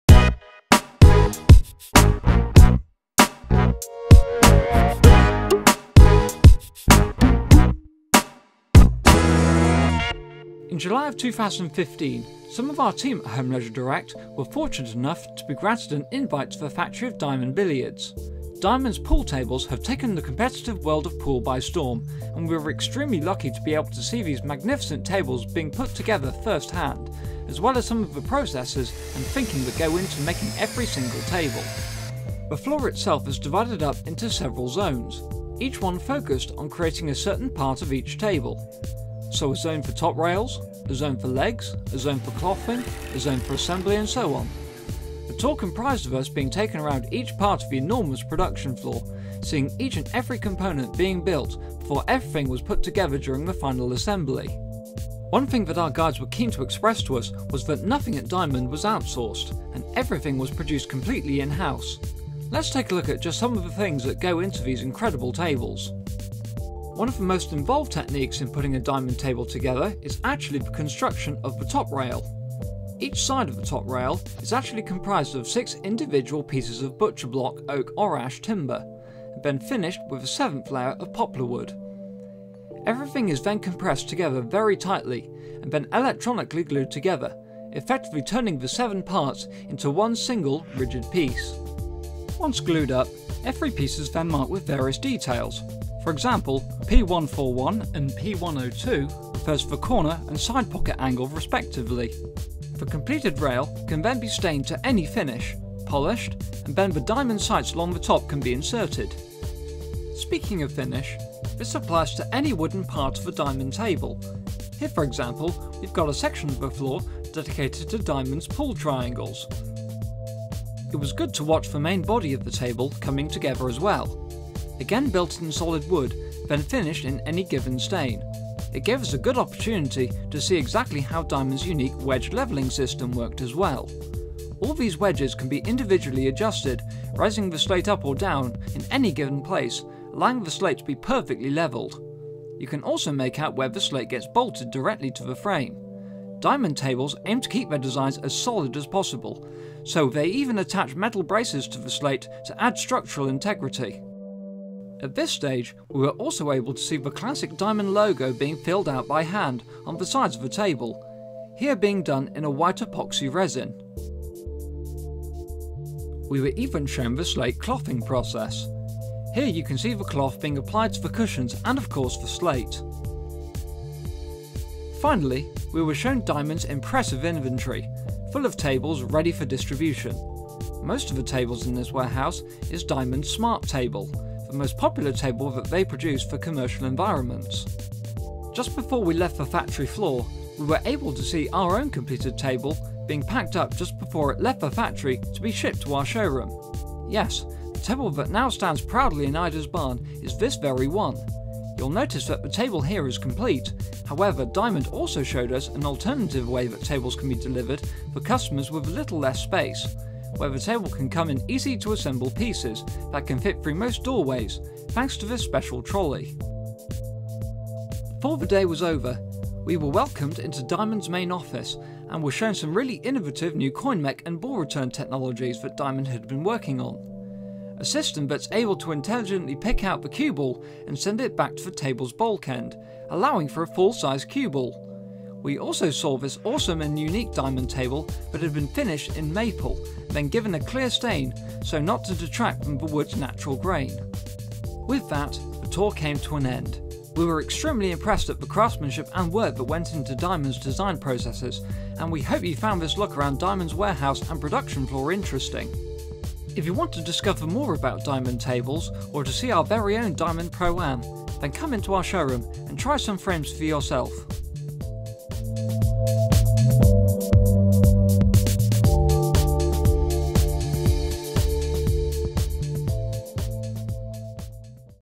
Diamond Billiards Factory Tour
In July of 2015, Diamond Billiards very kindly opened their doors to us, and allowed some of the team into their impressive factory in Jeffersonville, Indiana. The tour covered all of the factory floor, and we got to see Diamond's awesome 'smart table' being put together first hand. We even got to see our very own 9 foot Diamond Pro-Am pool table being packed up before its journey overseas to our showrooms!

To find out more about Diamond's tables, check out the Diamond Billiards section of our